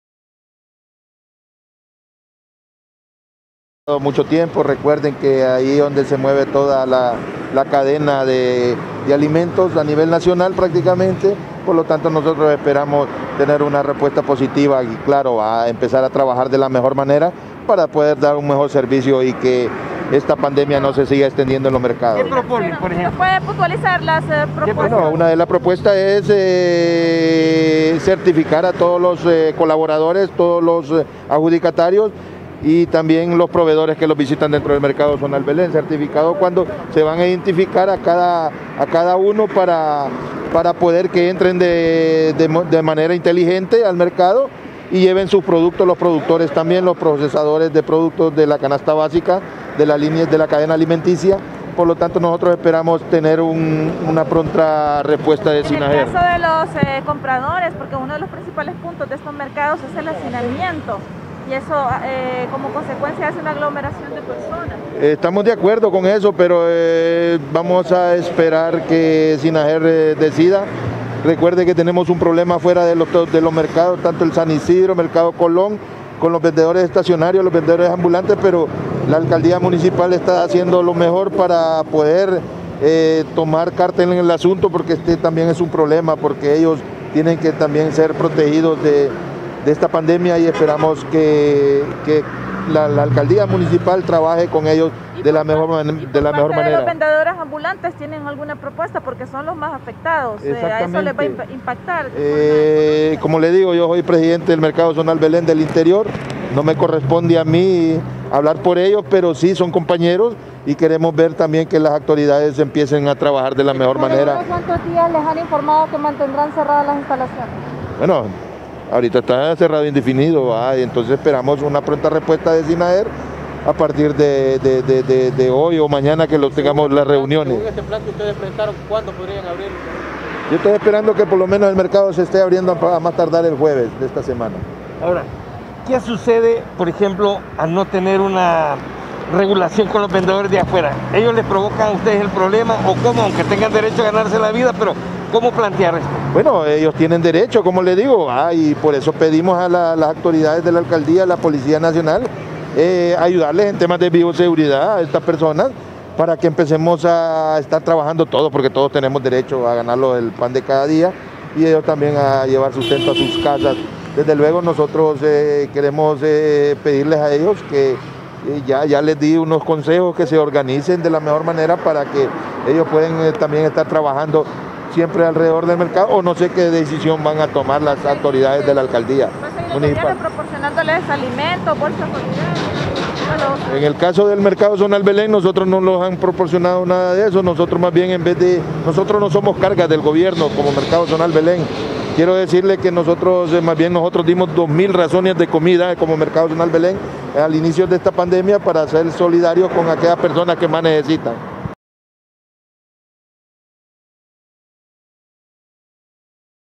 Dirigentes del Zonal Belén esperan reapertura el próximo jueves
El presidente de los vendedores del mercado Zonal Belén, Rafael Sánchez, declaró hoy que confían que puedan reiniciar operaciones el próximo jueves luego que se reúnan con Sinager a fin de buscar medidas de bioseguridad para beneficio de los locatarios.